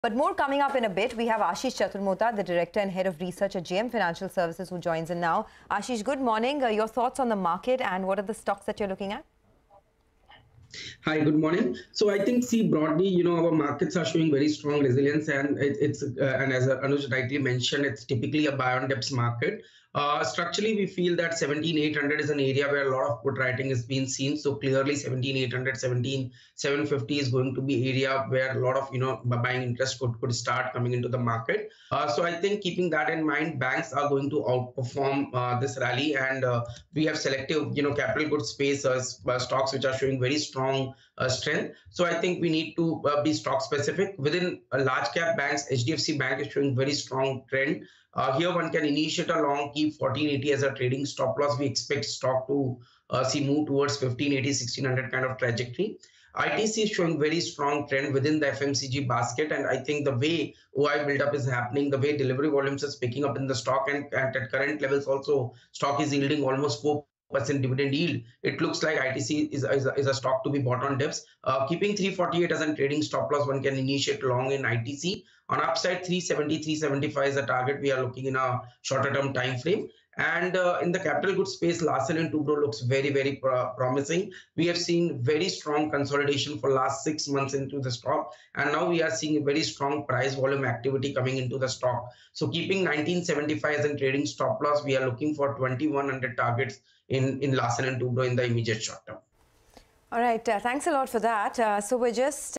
JM Financial's Ashish Chaturmohta's Technical Check On Markets & Key Stock Picks | CNBC-TV18
Ashish Chaturmohta, Director, Head Research, JM Financial Services, gives a technical check on markets and speaks on his key stock picks of the day. Listen in.

n18oc_business